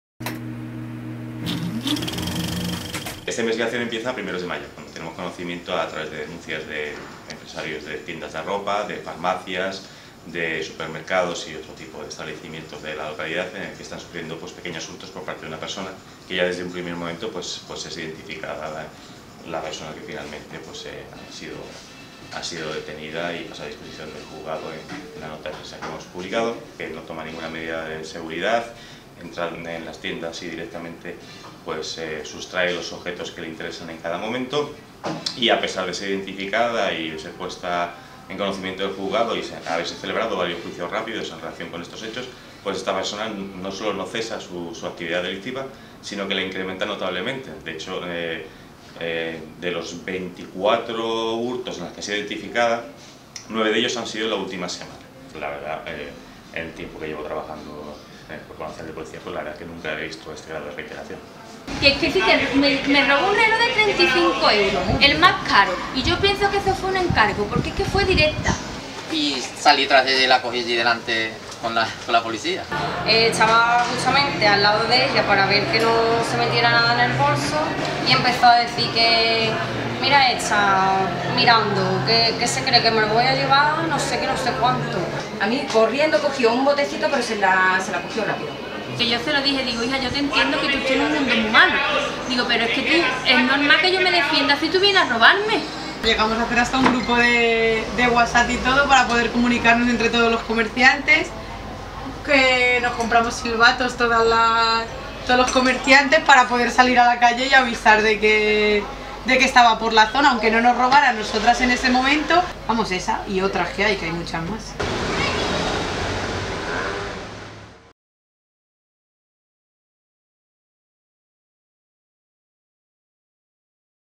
Sanlúcar respira tras el destierro de Cindy 'la ladrona'
Los comerciantes de la localidad sufrieron durante 40 días los continuos robos de una mujer a la que un juez impide pisar suelo sanluqueño.